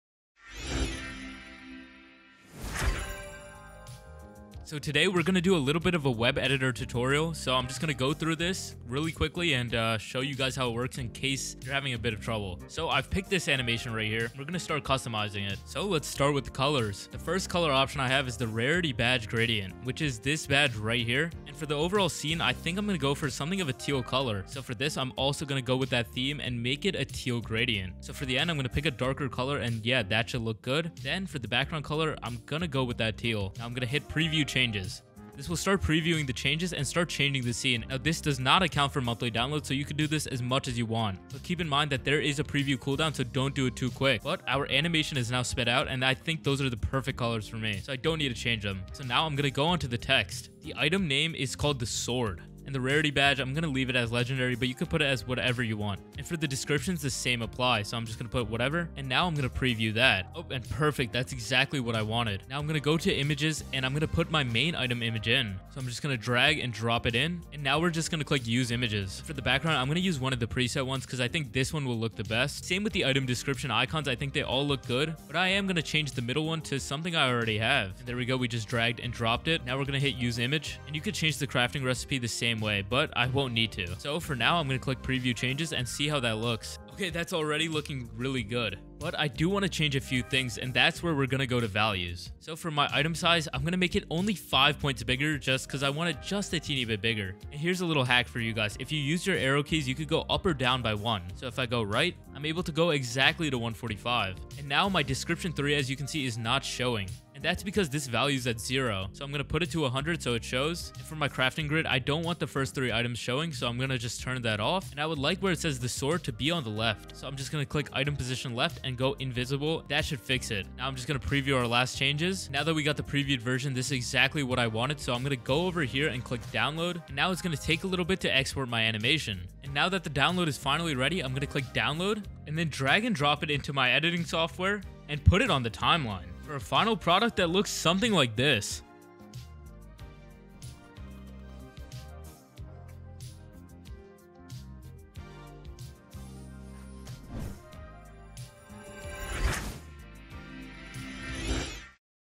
Making Minecraft Animations - ccLeaf Editor 3.0
Here is a short tutorial on how to make high quality After effects style motion graphics/animations using the ccLeaf 3.0 store. These sorts of edits can be seen from many popular youtubers such as @Sword4000 & @MindOfNeo